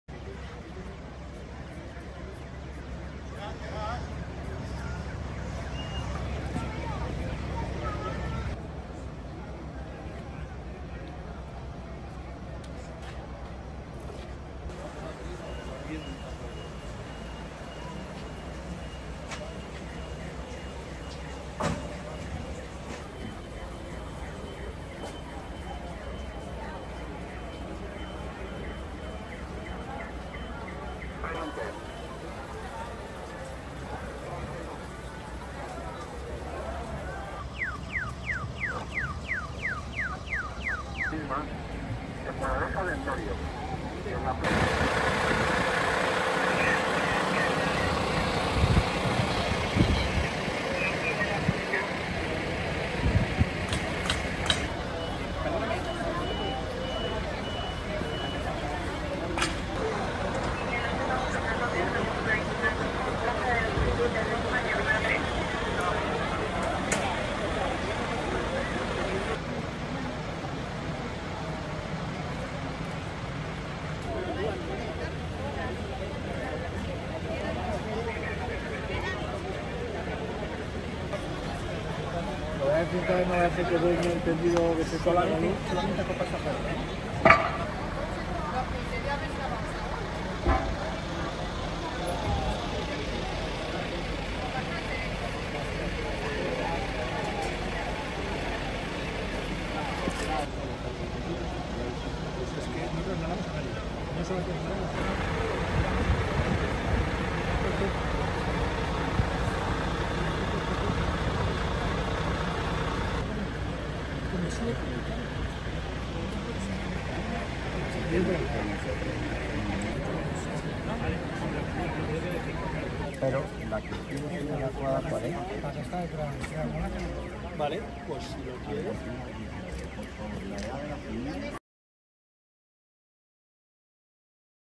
Una furgoneta choca contra un tranvía en Parla y provoca 17 heridos
El choque entre un tranvía y una furgoneta ha provocado un total de 13 personas heridas este sábado en Parla.
El siniestro, que ha terminado con 7 personas trasladadas por el Summa, ha tenido lugar en el cruce de la calle María Cristina con la calle Reyes Católicos a las 15:38 horas de este sábado por causas que ahora investiga la Policía Local, ha informado un portavoz de Emergencias Comunidad de Madrid 112.